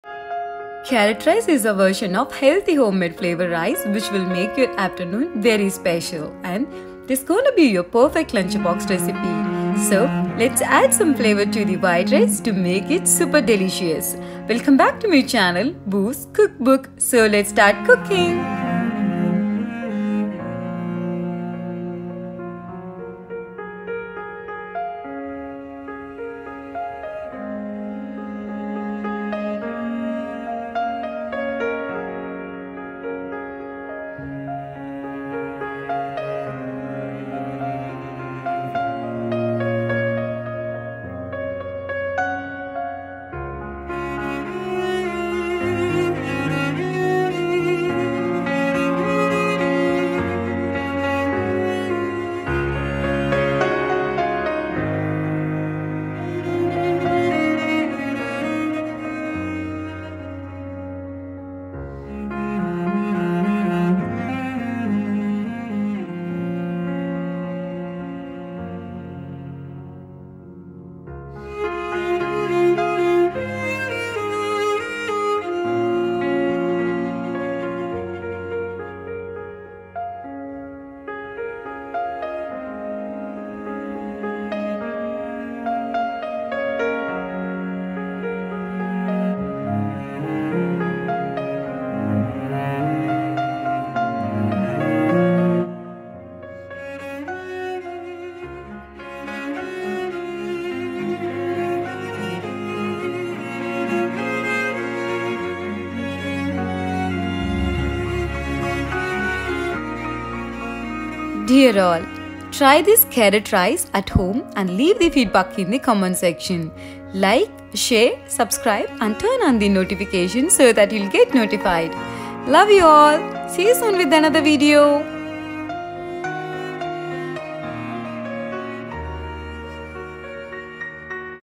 Carrot Rice | கேரட் சாதம் | Variety Rice | Lunch Box Recipe | Healthy Homemade Food | Mixed Rice
Ingredients

4 tablespoon of ghee
50g Peanuts
Ginger
2 onions chopped
5 green chilies
1/2 tablespoon of red chilli powder pinch of cumene powder
200g grated carrot
Sprig curry leaves
500g cashew nuts
2 cups of cooked rice

See you soon with another video.

Boo’s CookBook!